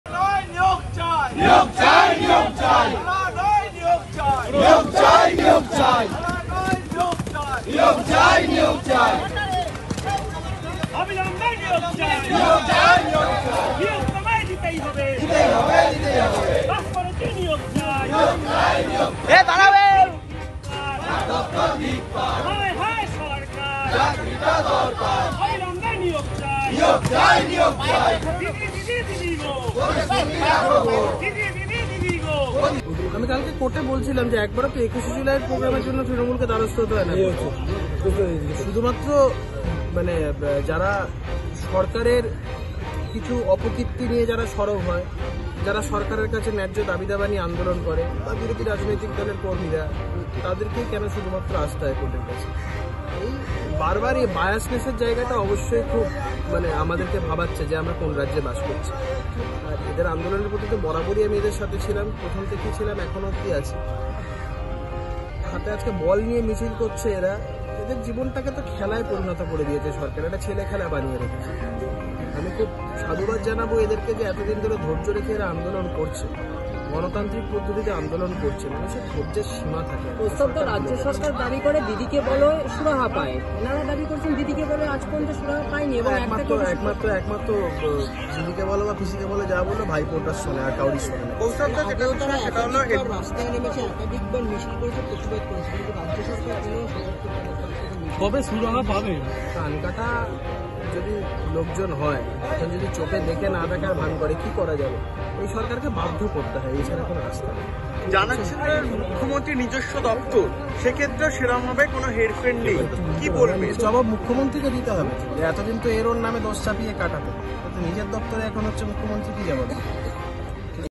Group D Procession : ‘দিদি দিদি দিদি গো... বলছি তুমি দেখো গো’ | Koustav Bagchi | Ei Samay
প্রথমে রাজ্য পুলিশের কাছে অনুমতি মেলেনি চাকরিপ্রার্থীদের মিছিল করার। ২১ জুলাই যেহেতু তৃণমূলের শহীদ দিবস রয়েছে, তাই তার দুদিন আগে অনুমতি দেওয়া যাবে না বলে শোনা যাচ্ছিল পুলিশের তরফে। শুরুতে এই মিছিলের অনুমতি কলকাতা পুলিশ না দেওয়ায় আদালতের দ্বারস্থ হয়েছিলেন আইনজীবী কৌস্তভ বাগচী (Congress Lawyer)। প্রাপ্য চাকরির দাবিতে ফের একবার পথে গ্রুপ ডি চাকরিপ্রার্থীরা। আদালতের রায়ে স্বস্তির মিছিল করল তাঁরা। তাঁদের স্লোগান ছিল, ‘দিদি দিদি দিদি গো... বলছি তুমি দেখো গো’। দেখুন সেই ভিডিয়ো।